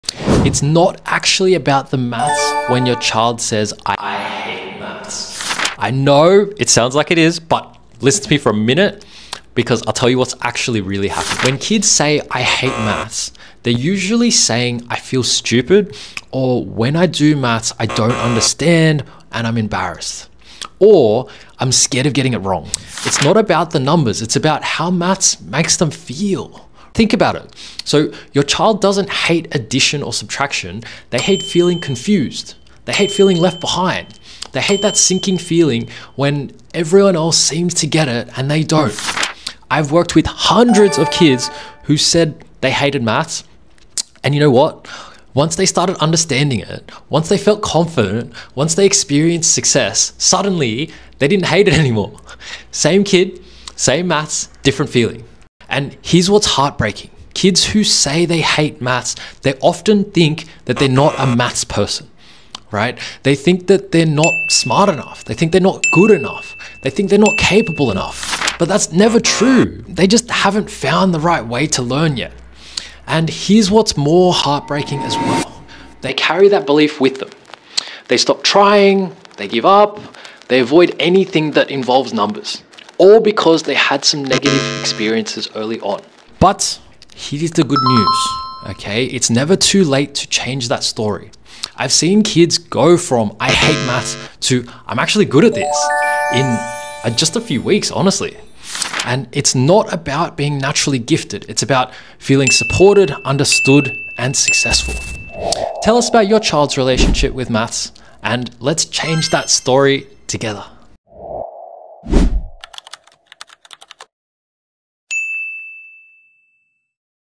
The Real Reason Kids Say “I Hate Maths” (And How to Fix It) | Lee Mathematics Tuition
Most kids don’t actually hate maths, they just hate the feelings that come with it. 😔

Confusion. Embarrassment. Feeling left behind. Thinking they're “not a maths person.”

In this video, we unpack the REAL reason children say “I hate maths,” and why it has nothing to do with intelligence.

Parents, kids don’t hate addition or subtraction, they hate the sinking feeling when everyone else “gets it” and they don’t.

But there's a good news...  ✨

With the right support, approach, and learning environment, kids stop hating maths and start believing, “Hey… I can actually do this.”

If your child is struggling, stressed, or saying “I’m just not a maths person,”  It's not yet the end. We can still turn this around. ❤️